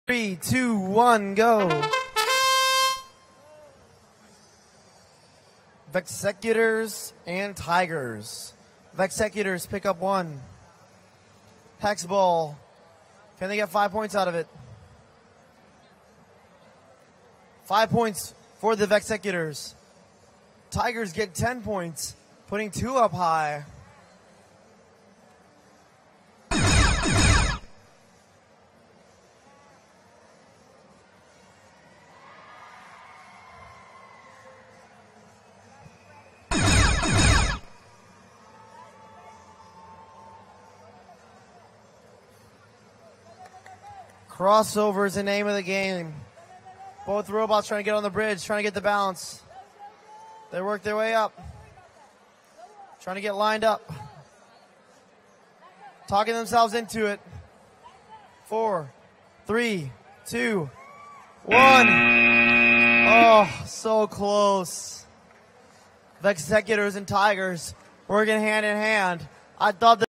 2017 VEXIQ Elem Sci Q50 (19105A 8615A) Score 30
VEX Worlds 2017 - VEX IQ Elementary School - Science Division - Qualifiers Match 50

Team Score 30:
19105A (TIGERS, Kendale Lakes Elementary School, Miami, Florida, United States)

8615A (The Vexecutors, New Hope Elementary School, Columbus, Mississippi, United States)